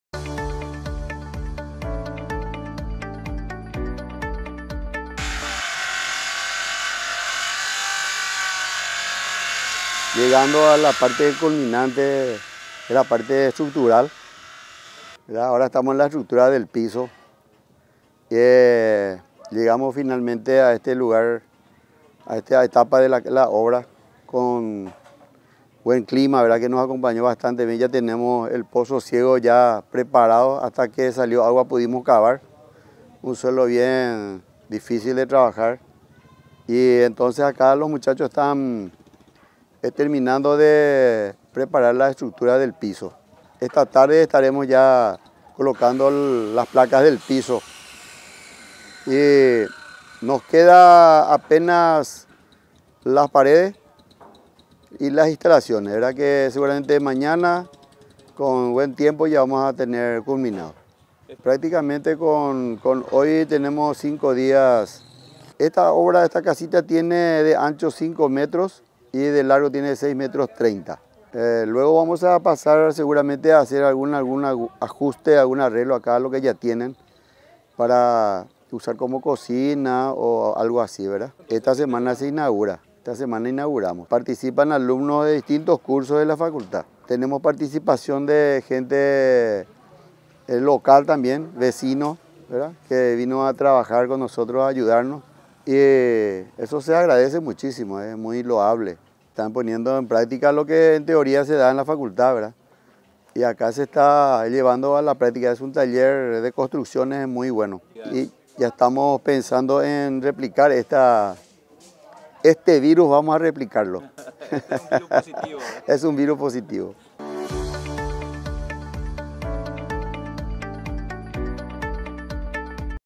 Felicidades al Arq. Félix Amarilla y a los voluntarios
Por esta obra social para beneficio de la señora Librada Martinez, del barrio San Blás de Pdte. Franco. Librada está con cáncer y es de escasos recursos. El Dpto. de Extensión universitaria de la UPE lleva adelante este trabajo social. La inversión es realizada por la Dirección Administrativa de la UPE, sede Pdte. Franco, a cargo del Lic. Luis Tiki González Vaesken.